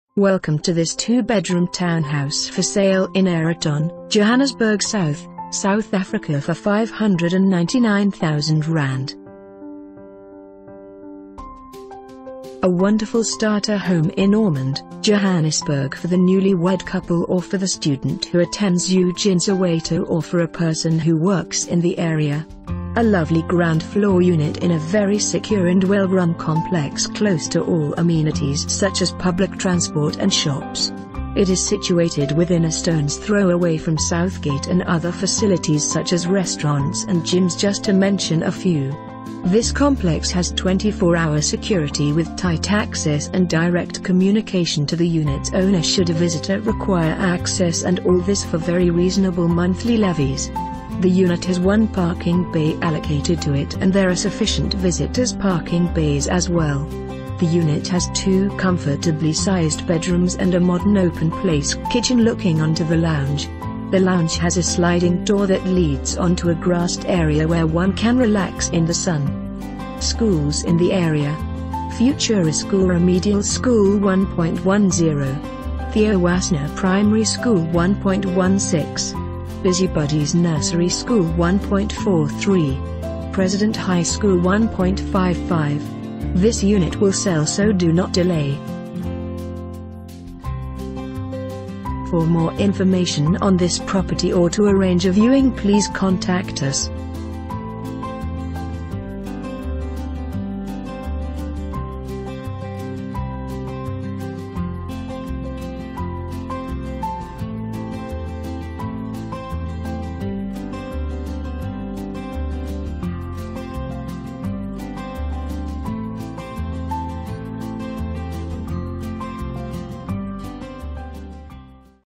2 Bedroom Townhouse For Sale in Aeroton, Johannesburg South, South Africa for ZAR 599,000...
To view videos of similar listings in Aeroton, Johannesburg, Gauteng please

Property Description

A wonderful starter home in Ormonde, Johannesburg for the newlywed couple or for the student who attends UJ in Soweto or for a person who works in the area. A lovely ground floor unit in a very secure and well run complex close to all amenities such as public transport and shops. It is situated within a stone’s throw away fro...